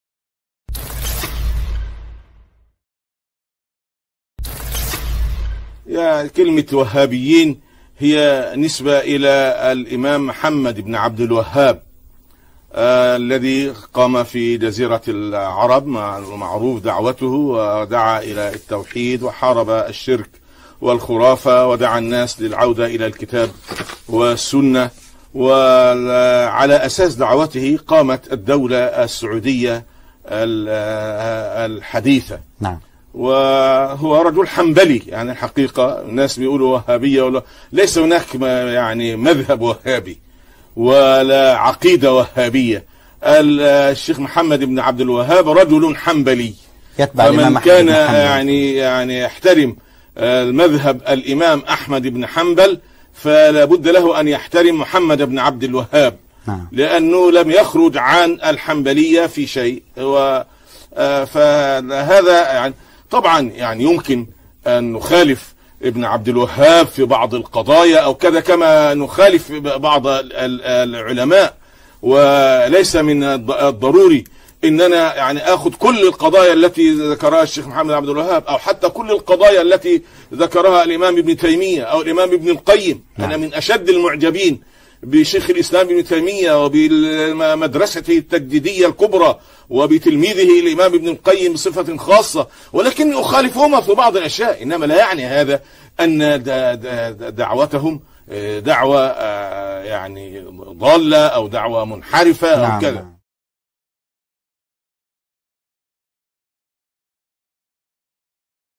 Siapakah Wahabi Itu? - Syaikh Dr. Yusuf Al-Qardhawi
Istilah Wahabi yang selama ini jadi momok penghinaan dan fitnah diantara ustadz dan dai, ternyata begini penjelasan Syaikh Dr. Yusuf Al-Qardhawi, salah seorang ulama besar Mesir (bukan Arab Saudi)